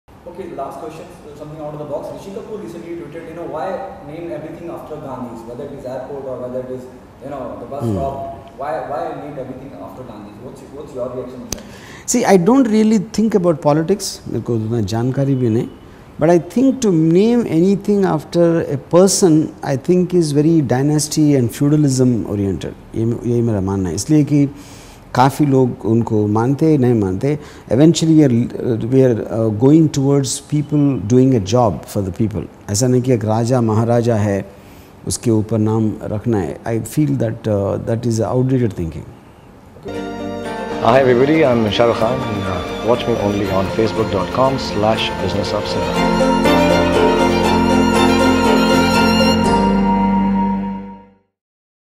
Ram Gopal Varma's Reaction On The Gandhis Controversy
Ram Gopal Varma speaks about the Gandhi controversy that is currently crucial debate going in the country. Watch the video to know more.